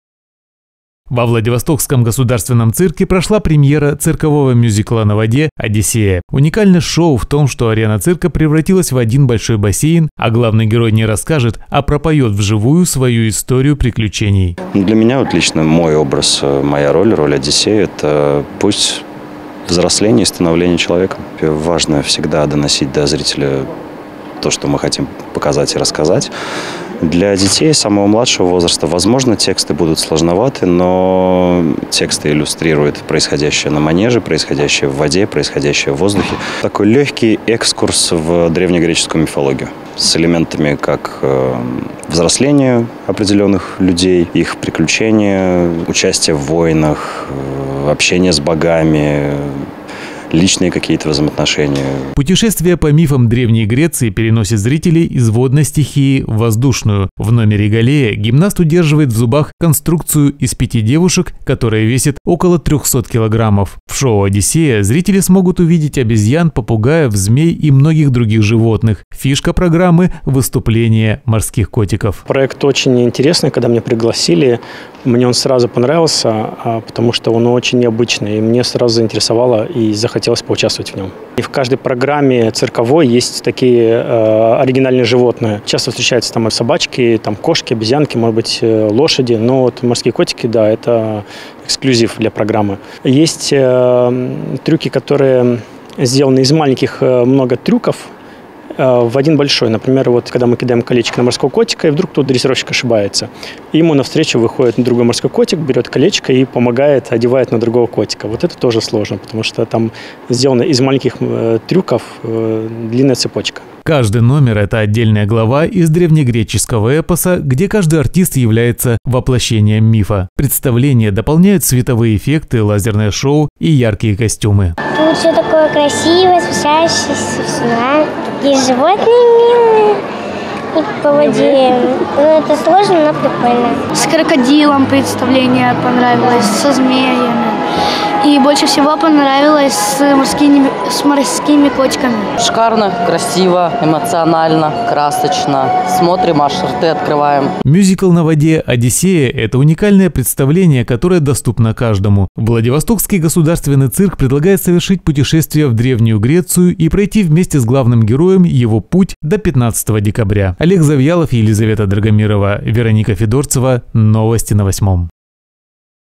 Грандиозная премьера – цирковой мюзикл на воде «Одиссея»
Во Владивостокском государственном цирке прошла премьера «Циркового мюзикла на воде «Одиссея». Уникальность шоу в том, что арена цирка превратилась в один большой бассейн, а главный герой не расскажет, а пропоёт вживую свою историю приключений.